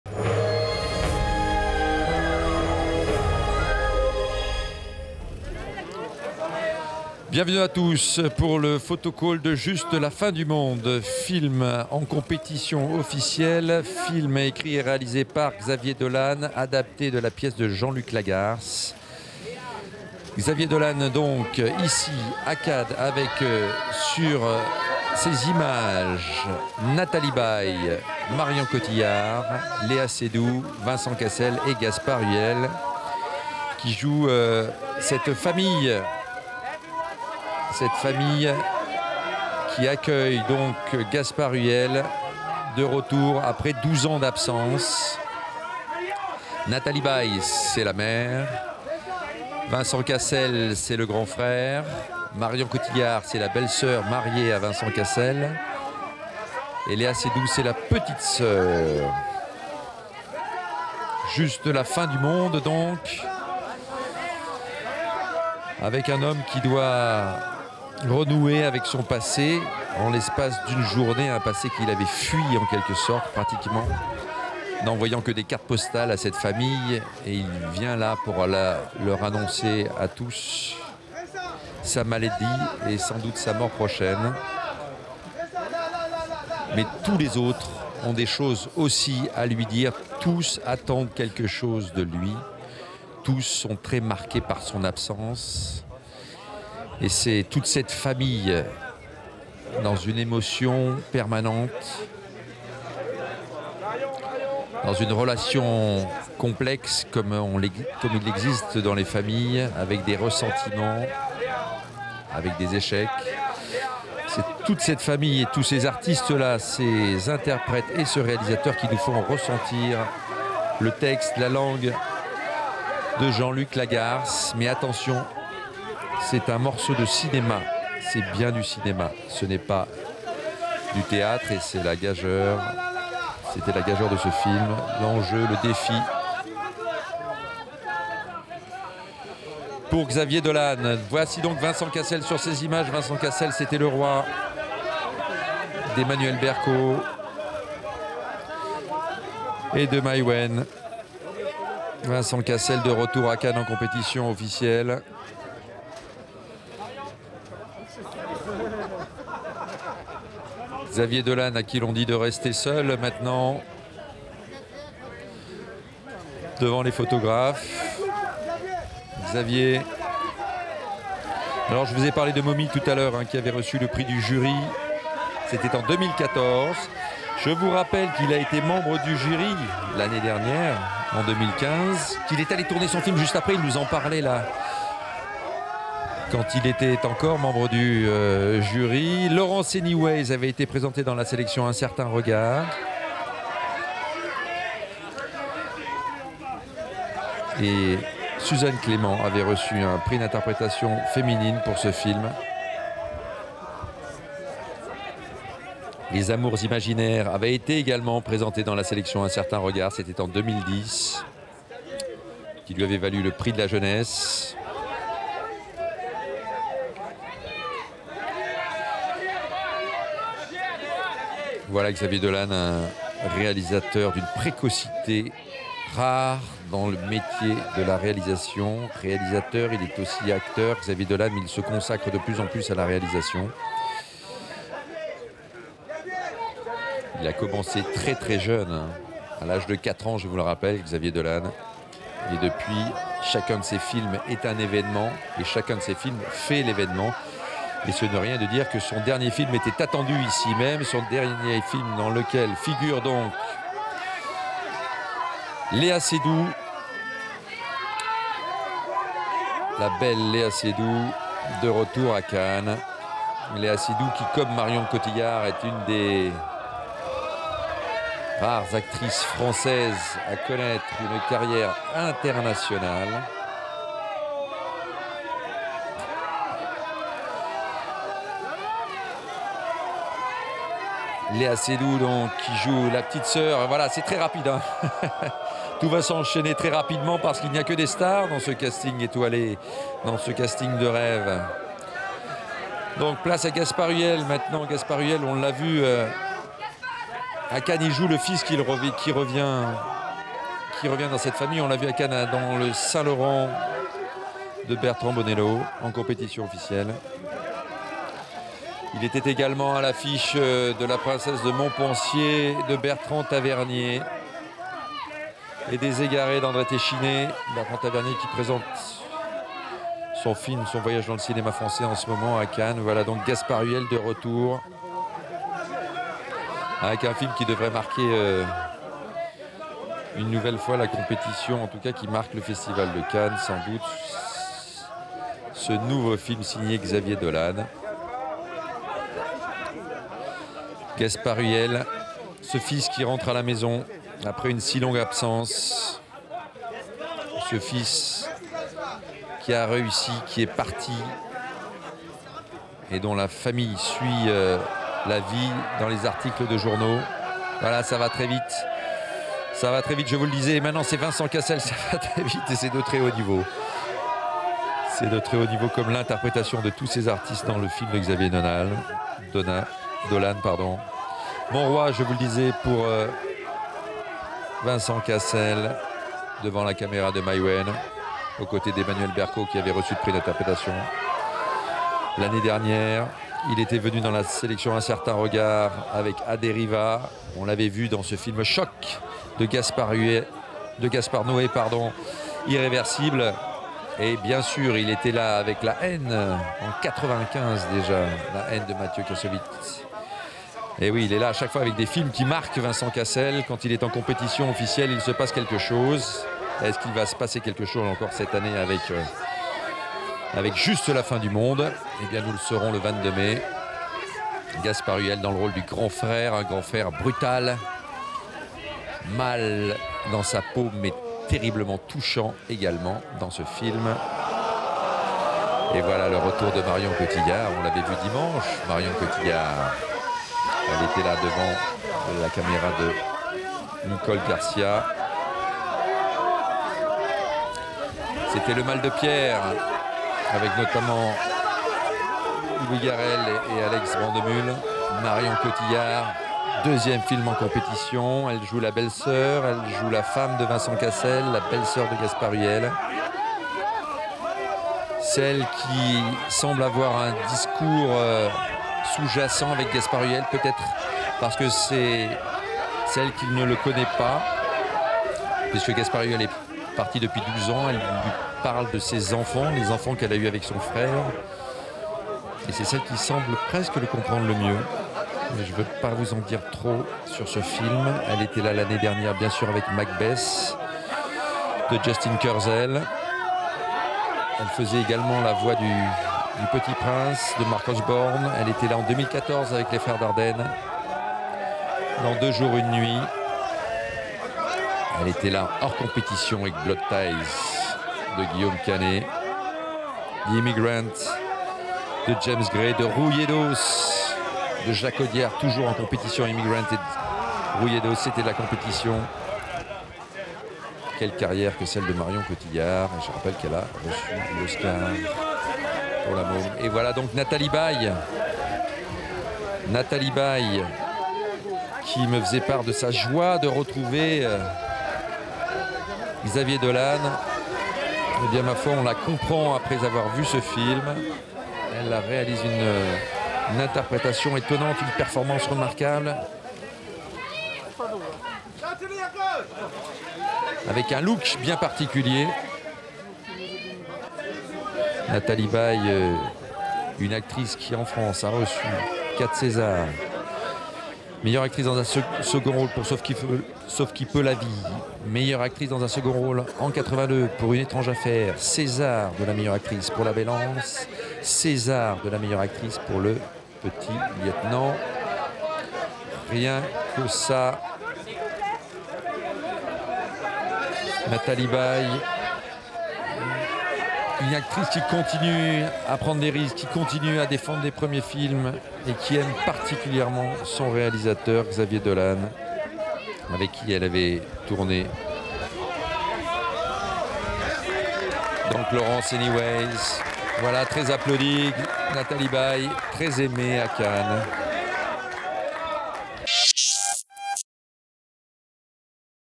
JUSTE LA FIN DU MONDE - Photocall - VF - Cannes 2016
Photocall JUSTE LA FIN DU MONDE : Xavier Dolan, Marion Cotillard, Gaspard Ulliel, Lea Seydoux, Vincent Cassell, Nathakie Baye